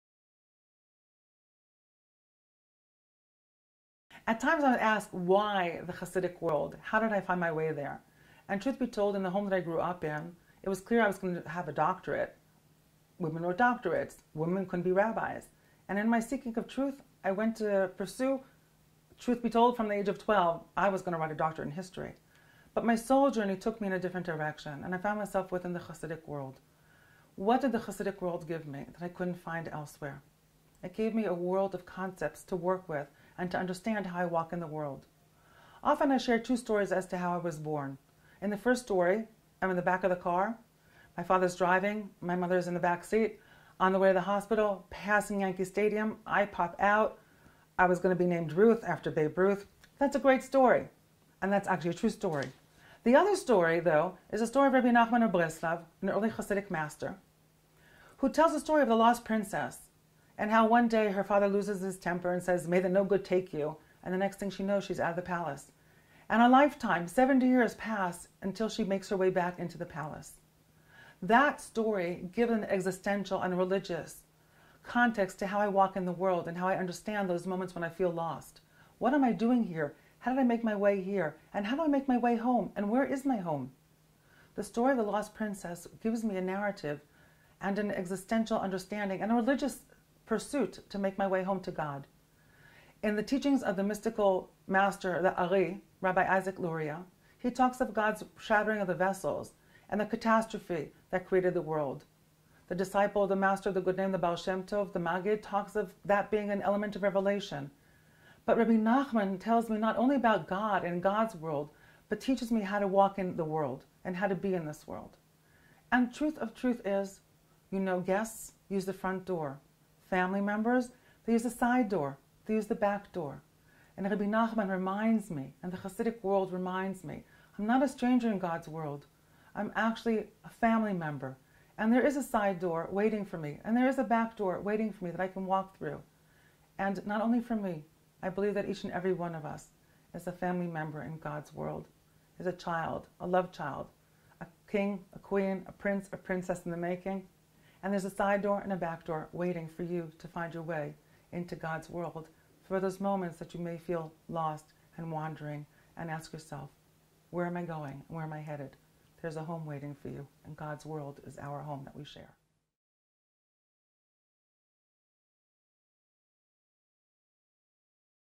How Do You Know When You Are Home? Rabbi Reb Dr. Mimi Feigelson on Hasidism
What is the meaning of finding your home? Sometimes it is knowing your story of origin and sometimes it is locating your spiritual roots. Rabbi Dr. Reb Mimi Feigelson, Mashpiah Ruhanit (Spiritual Mentor) and senior lecturer of Rabbinics and Hasidic Thought at the Schechter Rabbinical Seminary, discusses how she first became interested in Hasidic Thought.